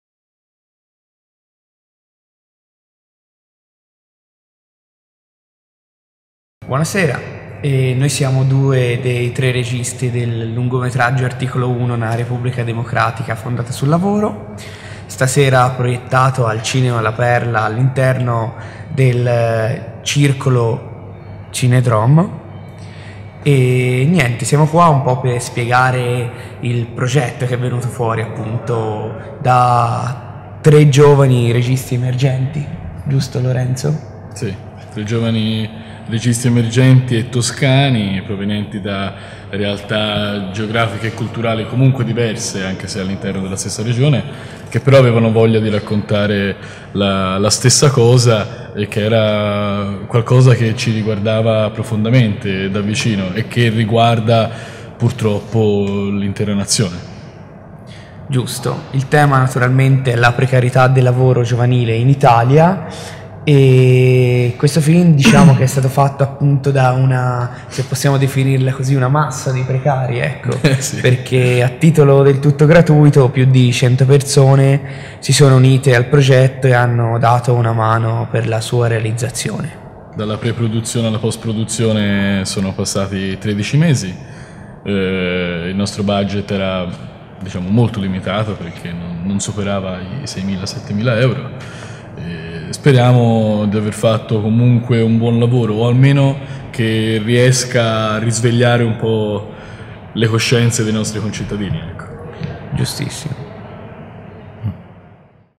ARTICOLO 1 - Intervista agli autori Daniele Trengia e Lorenzo Carcasci a Empoli 2012
CineDrome Cinema La Perla Empoli - gio 26 aprile 2012 - evento speciale alla presenza degli autori intervista agli autori di
ARTICOLO 1 - UNA REPUBBLICA DEMOCRATICA FONDATA SUL LAVORO di Adriano Davi - Lorenzo Carcasci - Daniele Trengia
Il progetto è nato dall'idea di 3 ragazzi toscani appassionati di cinema, Adriano Davi (Prato), Lorenzo Carcasci (Firenze) e Daniele Trengia (Viareggio) che, uniti dall'interesse per le tematiche socioculturali e la comunicazione visiva, hanno deciso di raccontare storie di vita vissuta nell'Italia dei 150 anni. La produzione è Dreamcommunication, nello specifico i produttori sono Andrea Colato e Daniele Trengia. Il film è stato girato in toscana nelle 3 città natali dei registi tra giugno e luglio 2011 e sono già state fatte 4 proiezioni al multisala Adriano a Firenze dove è stato fatto soldout tutte le volte